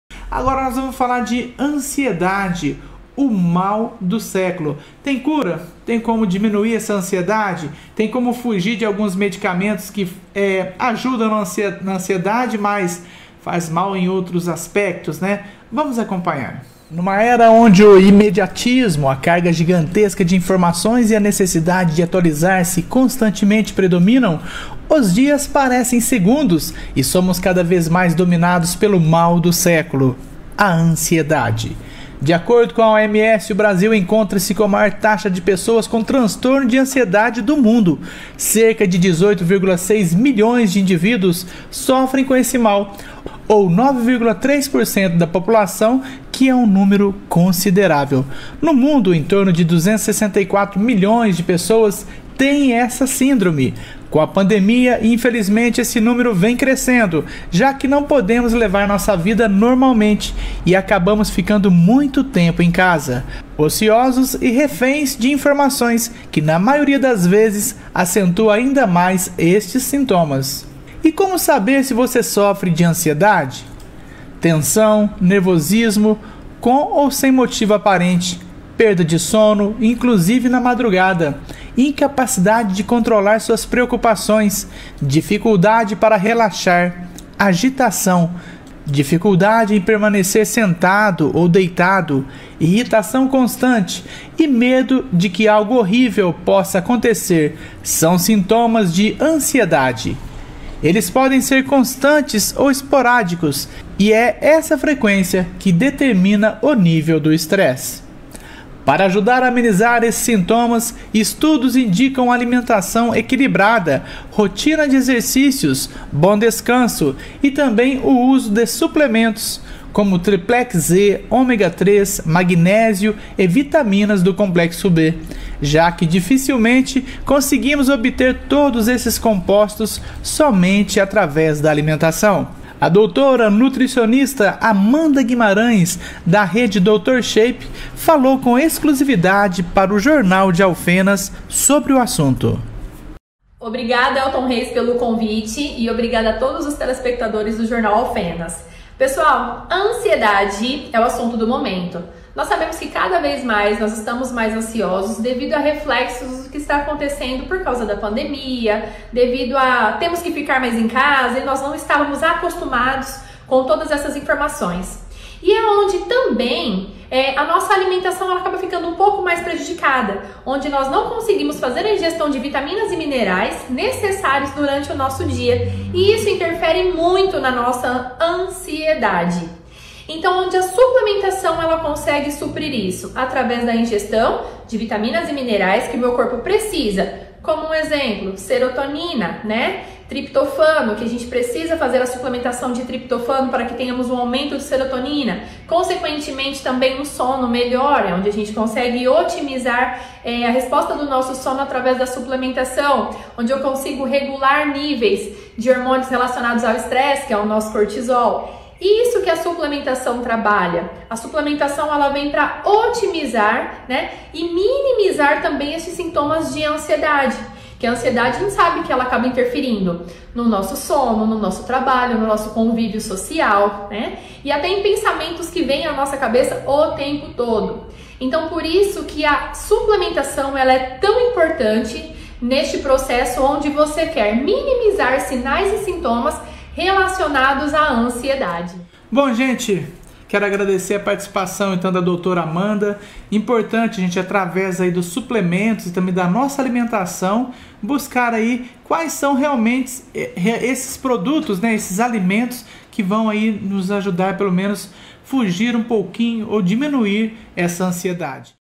Ansiedade: Como combater o mal do século?
Amanda Guimarães é nutricionista e explica como podemos mudar essa situação através de uma alimentação correta e uso de suplementos!

TV Grava - Um novo jeito de ver TV!
para receber as notificações de vídeos novos!